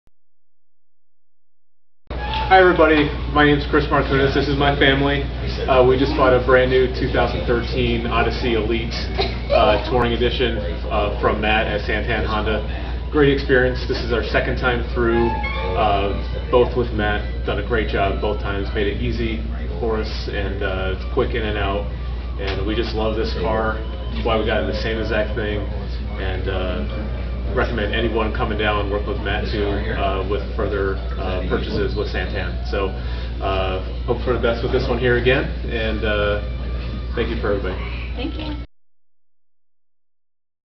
SanTan Honda Sales Testimonial - Chris and family 2013 Odyssey
- Chris and family had a great experience when they bought their 2013 Honda Odyssey.